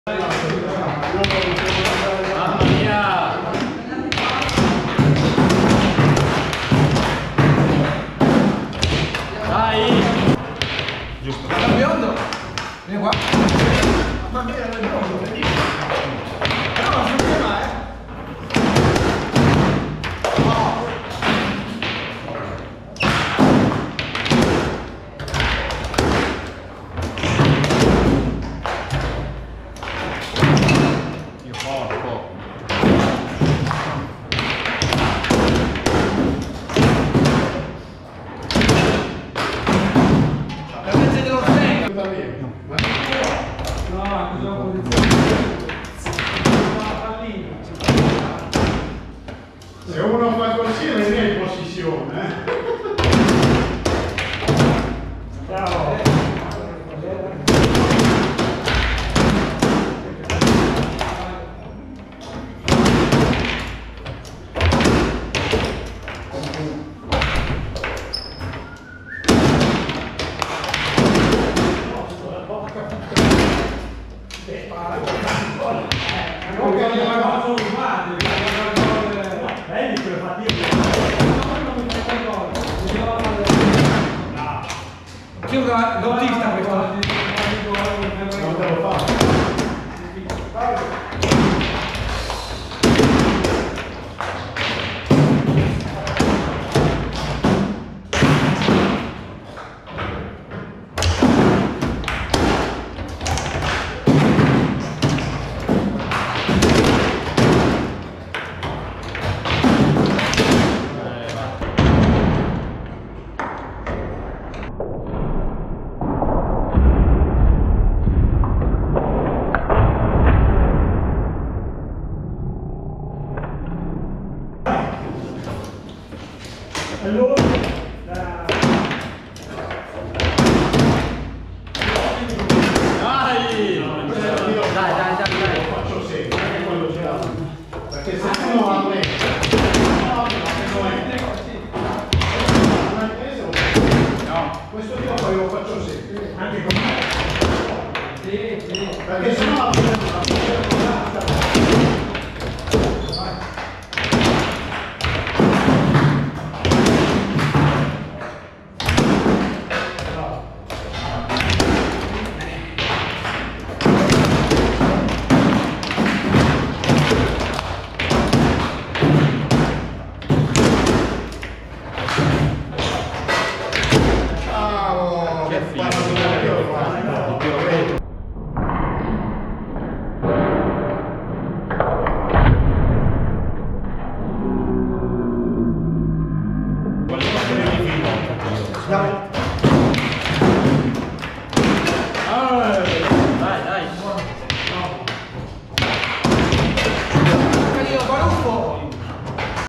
Calcio Balilla - Spanò/Fiore vs Cornacchione/Cucchi (reg. rollerball).
Partita ai 7 giocata su calcio balilla frimm super faster;
Regolamento tre tocchi rollerball licb;

* Stecche Blu:
Difesa: Fiore Ciro
Attacco: Spanò Cristian

* Stecche Rosse:
Difesa: Cucchi Alberto
Attacco: Cornacchione Roberto

Video effettuato con Gopro hero 10 black
Risoluzione 1920 x 1080 a 30 fps